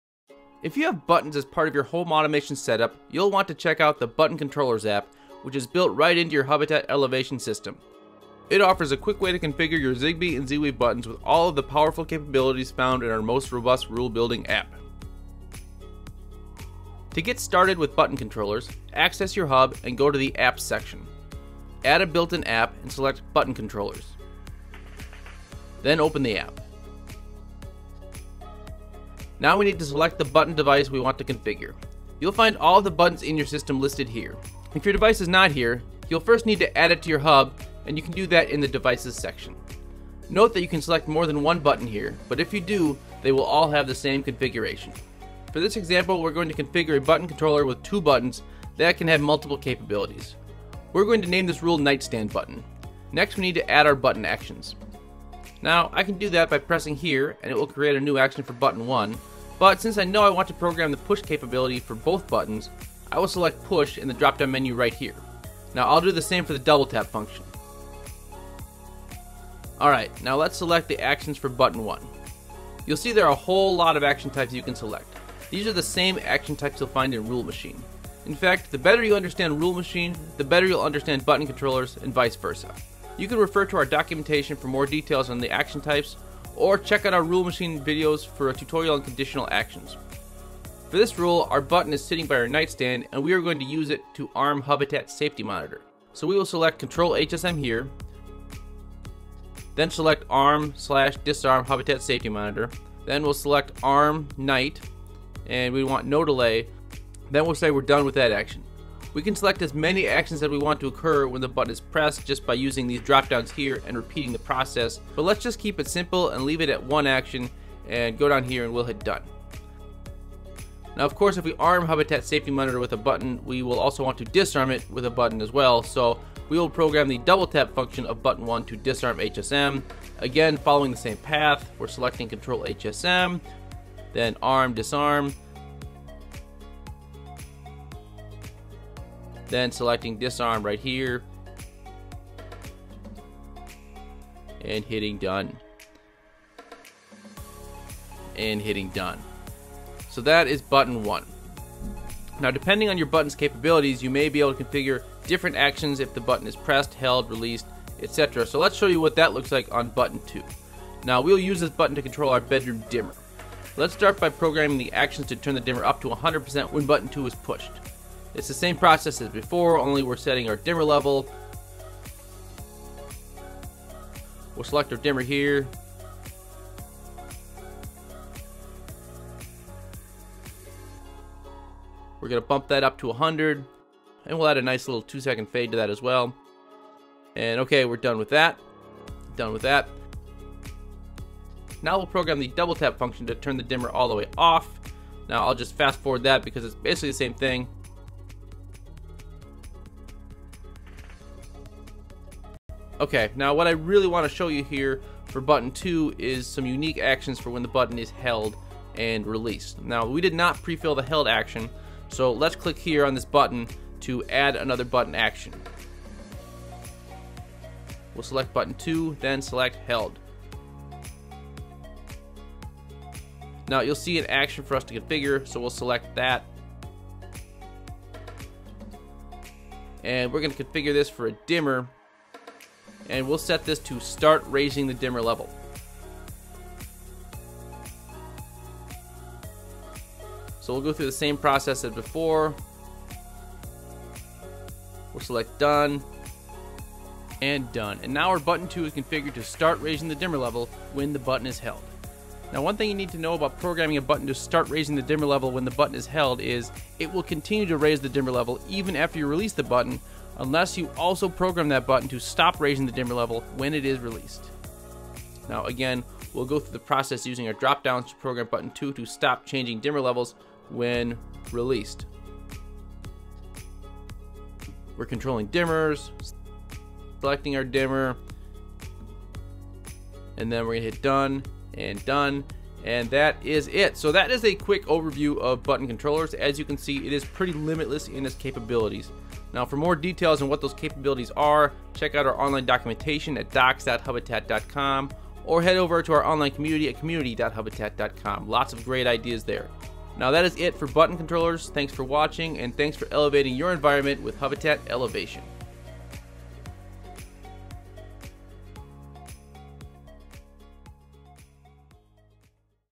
How to use button controllers app to automate buttons with the Hubitat Elevation hub
This video tutorial will show you how to set various programmable buttons using the Button Controllers App in the Hubitat Elevation home automation hub. Hubitat Elevation is compatible with most Z-Wave and Zigbee button controllers and other popular buttons such as Lutron Pico buttons (requires the Lutron Caseta Pro bridge)